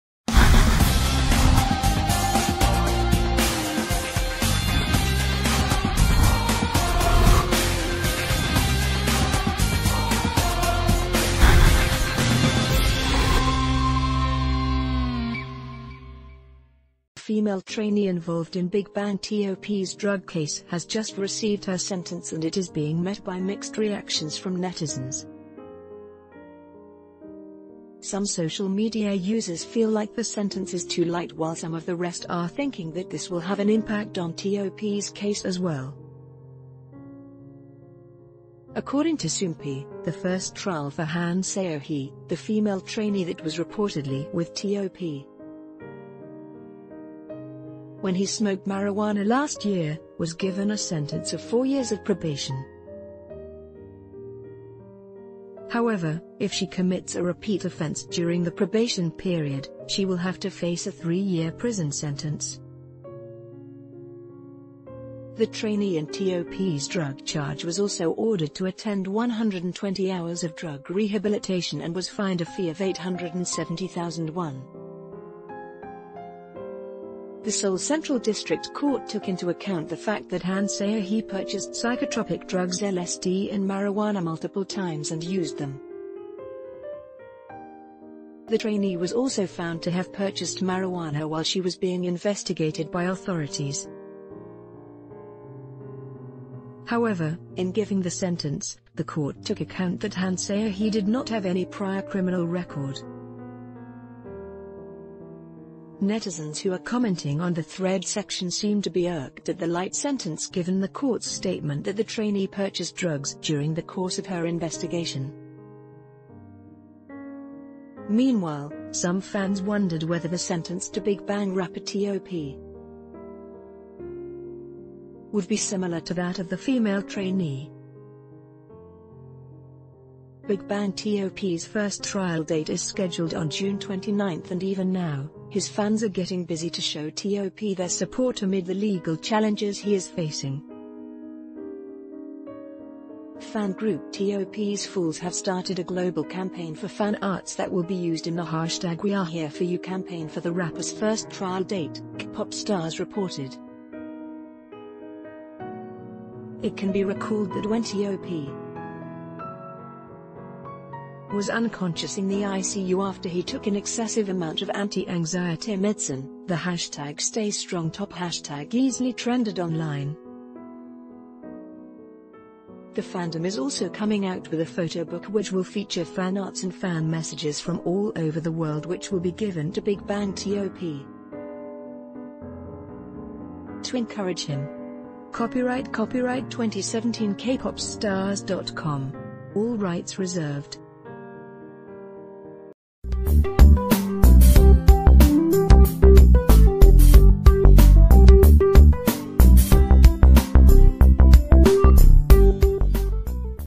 Sentencing For Female Trainee Involved In BIGBANG T.O.P's Case Draws Flak From Netizens
The female trainee involved in BIGBANG T.O.P's drug case has just received her sentence and it is be...